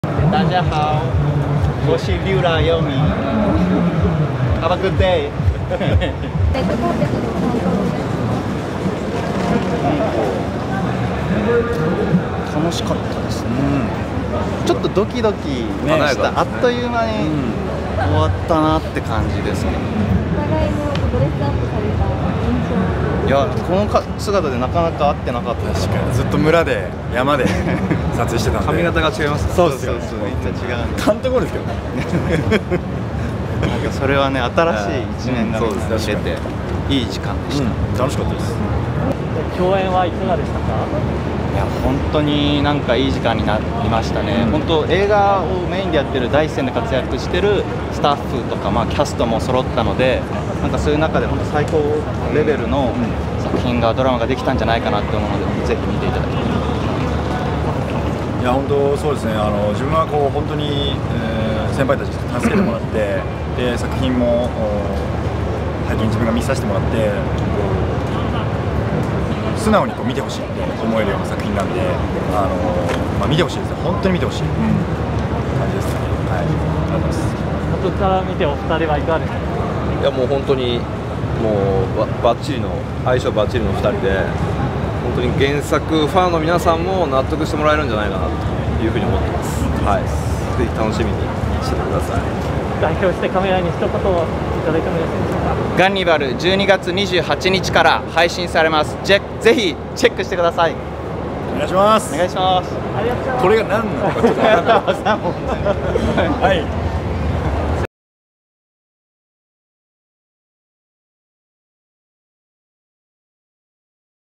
柳楽優弥＆笠松将、お互いのスーツ姿に照れ笑い レッドカーペットは「ドキドキした」第35回東京国際映画祭 オープニング『レッドカーペット』
日本発ディズニープラス「スター」オリジナルドラマシリーズ『ガンニバル』(12月28日より配信)で主演を務める俳優の柳楽優弥が24日、東京・日比谷で行われた「第35回東京国際映画祭」(TIFF)のレッドカーペットに登場した。

（記事＆写真）
📸柳楽優弥「最高のドラマができました」 東京国際映画祭で主演ドラマをアピール

🎬柳楽優弥&伊藤沙莉、アドリブ合戦で笑いが止まらず！？SmartNews新CM「SmartNewsニュース篇」メイキング&インタビュー公開